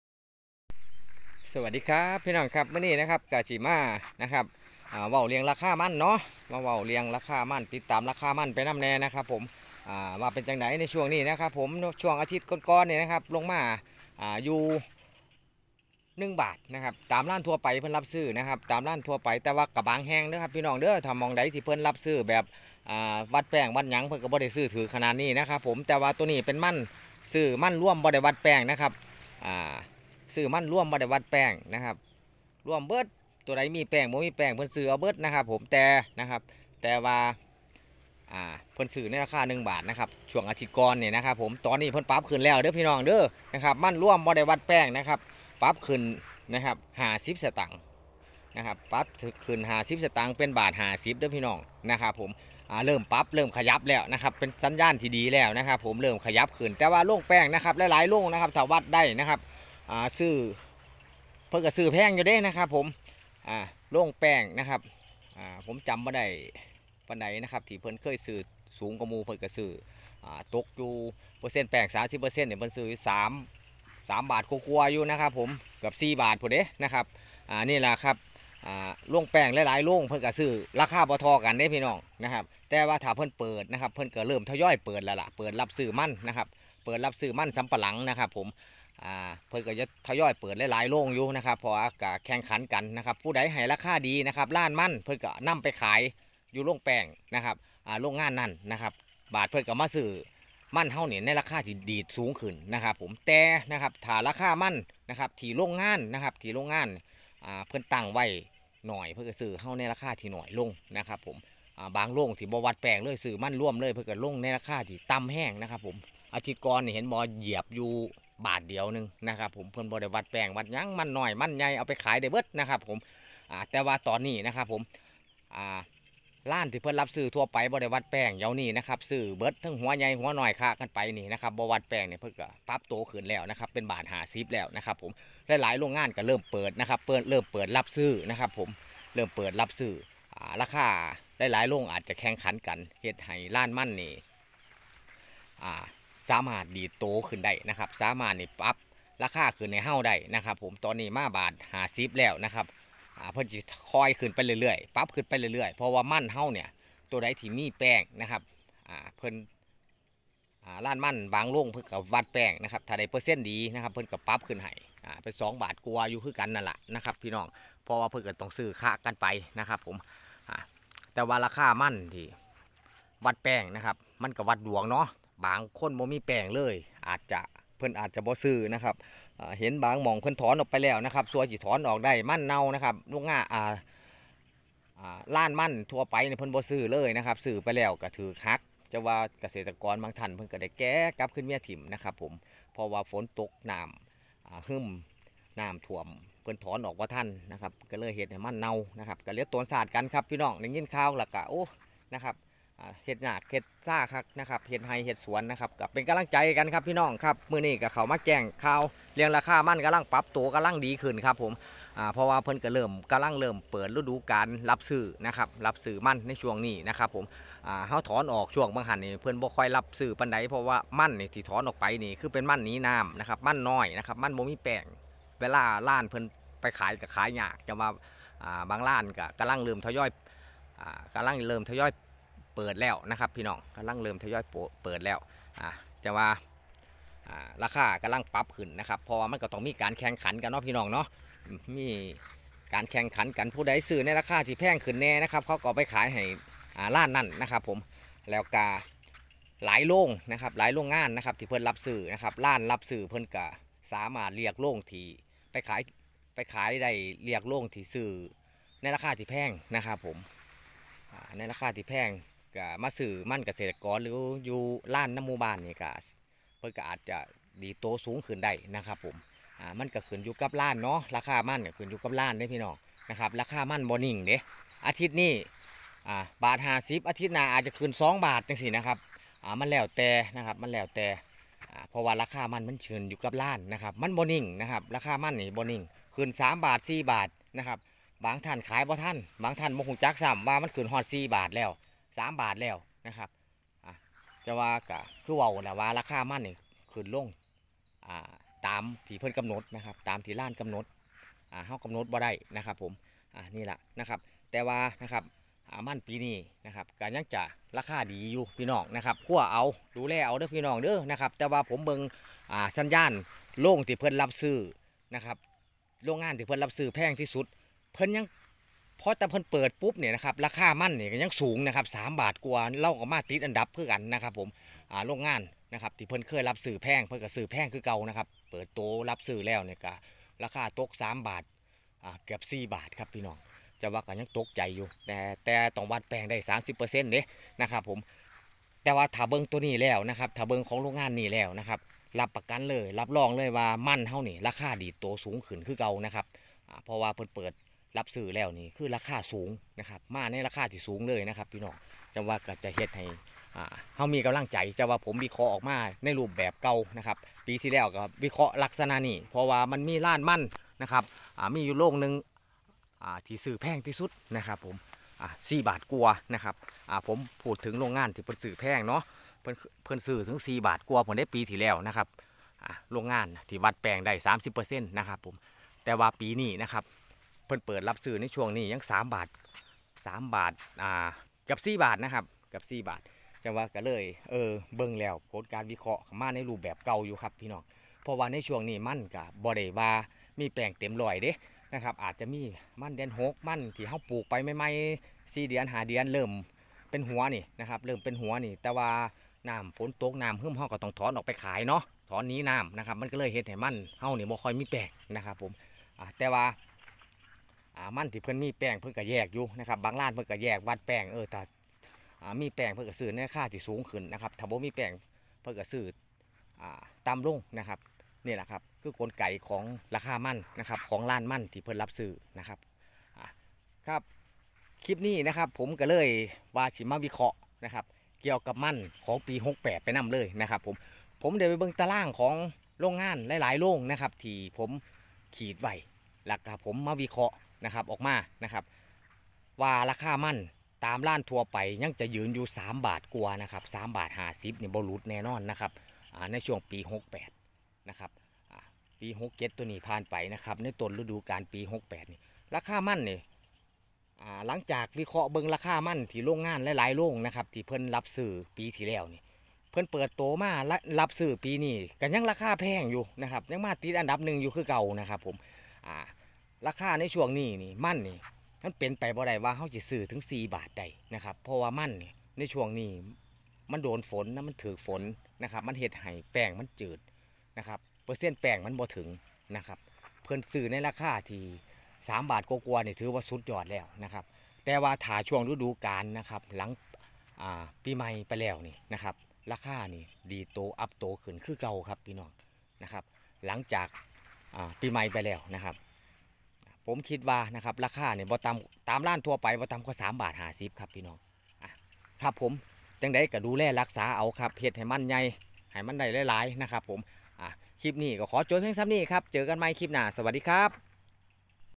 ราคามันเริ่มปรับขึ้นแล้ว หลายโรงงานรับซื้อ3บาทกว่า เป็นสัญญาณที่ดีราคามันปี68ยังสูงครับ
ราคามันเริ่มปรับตัวสูงมันปี68ราคาดีครับ#มันสำปะหลัง